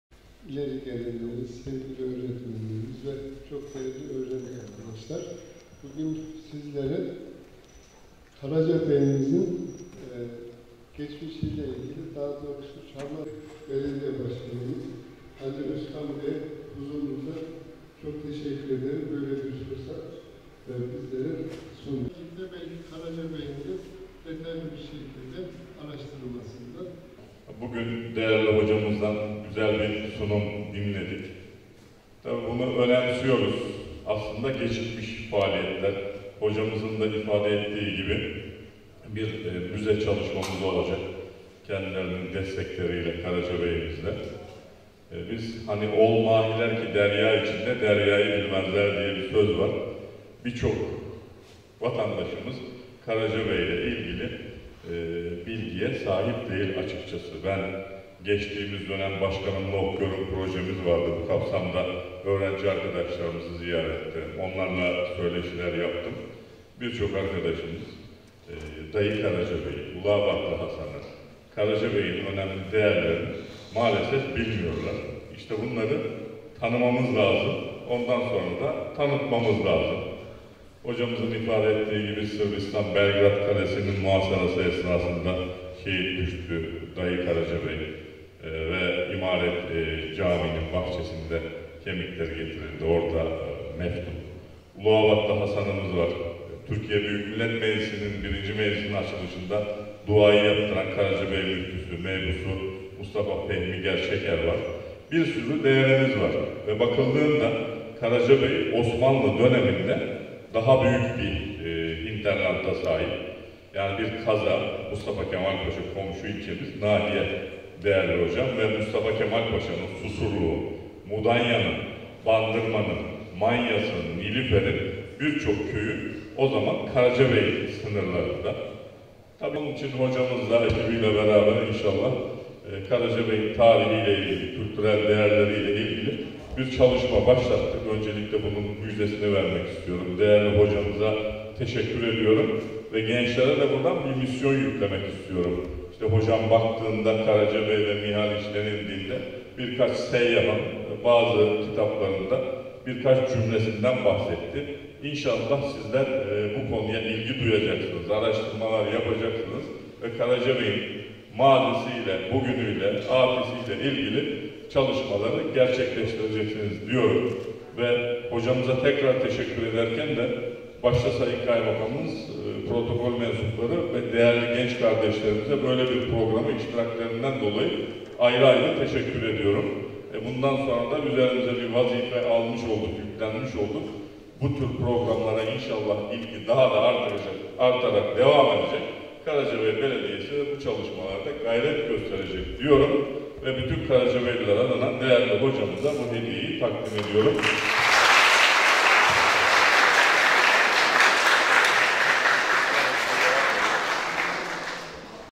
Geçmişten bugüne Karacabey konuşuldu
Karacabey Belediyesi tarafından, ‘Karacabey’in düşman işgalinden kurtuluşunun 100. yılı’ etkinlikleri kapsamında önceki gün düzenlenen ‘Çağlar Boyunca Karacabey’ sunumu, protokol üyeleri ve öğrenciler tarafından beğeniyle karşılandı.